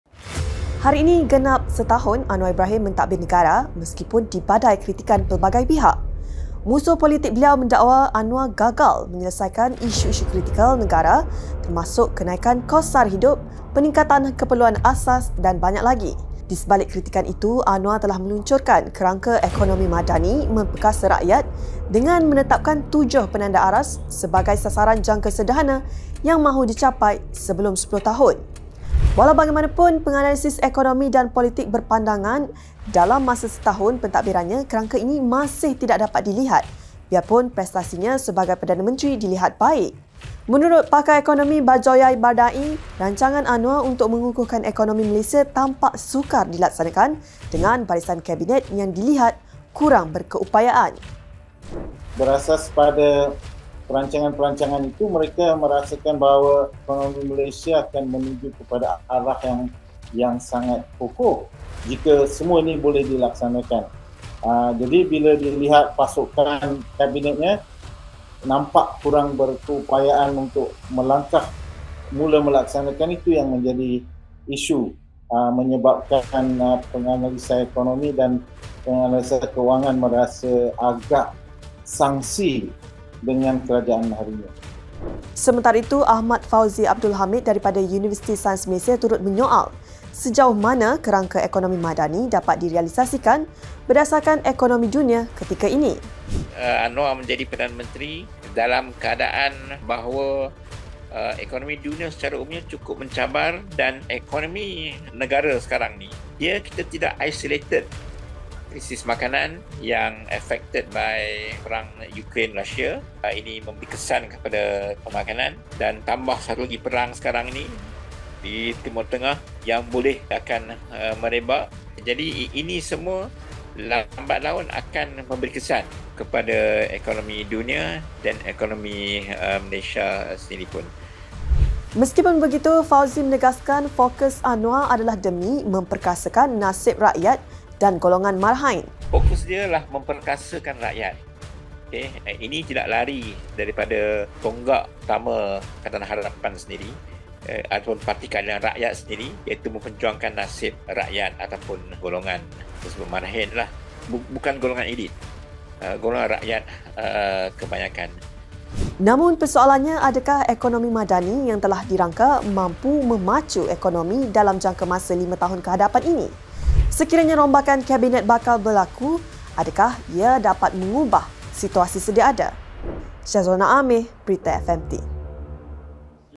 Setahun pentadbiran Anwar: Ke mana hala tuju Ekonomi Madani?
Hari ini genap setahun Anwar Ibrahim mentadbir negara biarpun beliau dikritik kerana gagal menyelesaikan beberapa masalah negara termasuk harga barang.